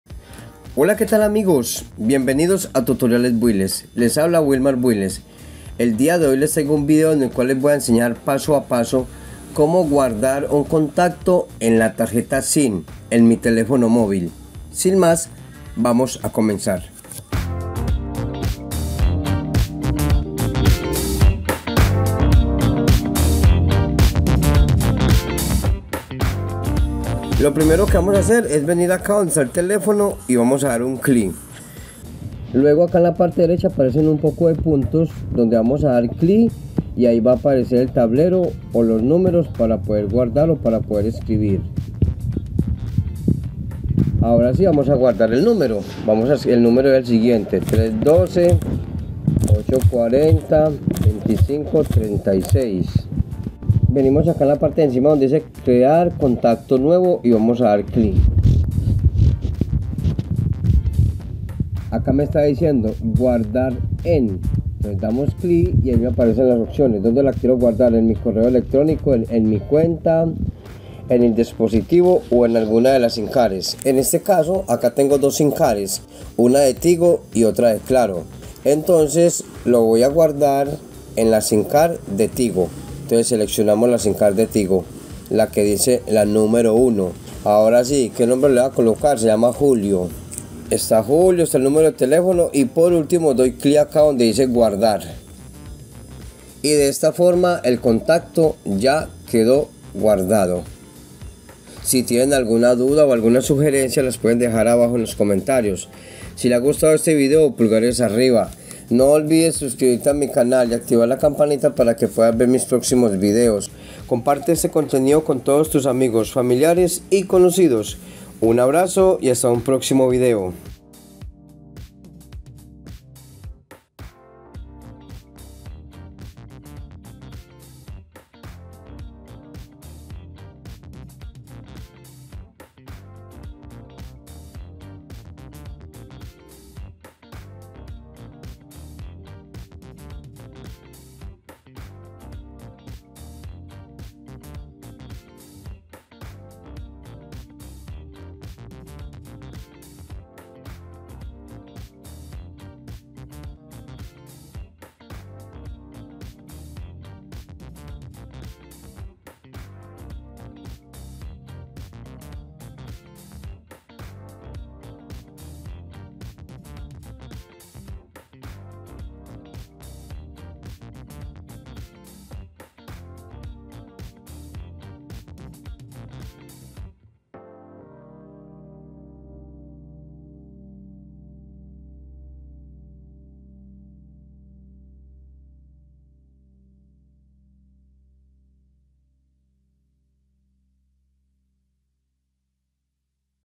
Cómo guardar contactos en la tarjeta sim de Mi Teléfono móvil / Fácil y rápido
En esta ocasión les estaré enseñando, cómo pasar o guardar los contactos en la tarjeta sim de mi teléfono  móvil.
Espero que este contenido sea de mucha utilidad para todos, si tienen dudas o sugerencias las pueden dejar abajo en los comentarios.